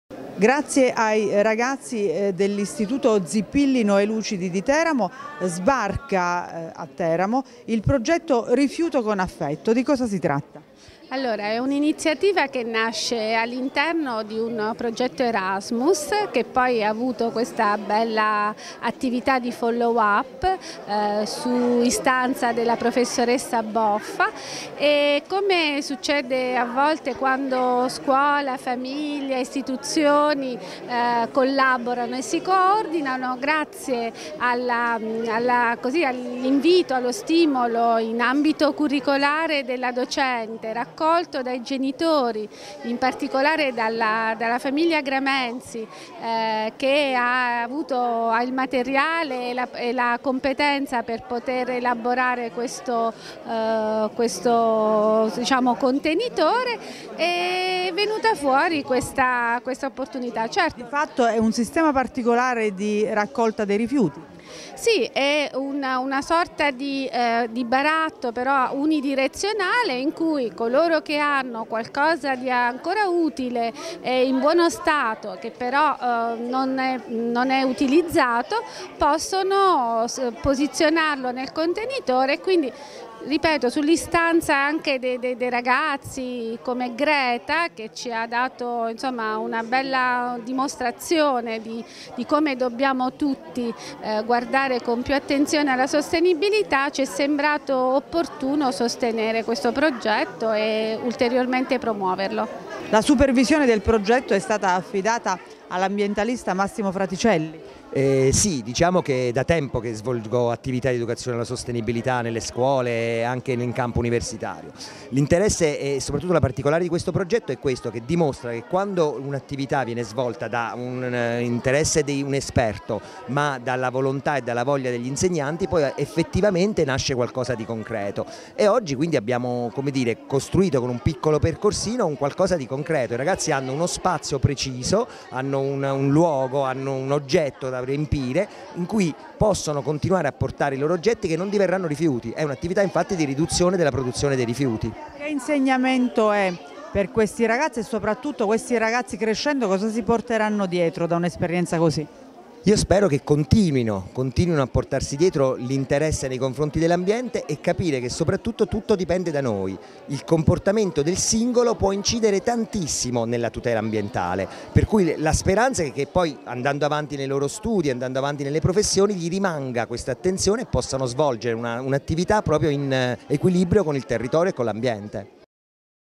Teramo, presentato all'I.C. ZIppilli-Lucidi il progetto "Rifiuto con affetto"
| SUPER J |

Contatti Redazione:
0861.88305

Seguiteci su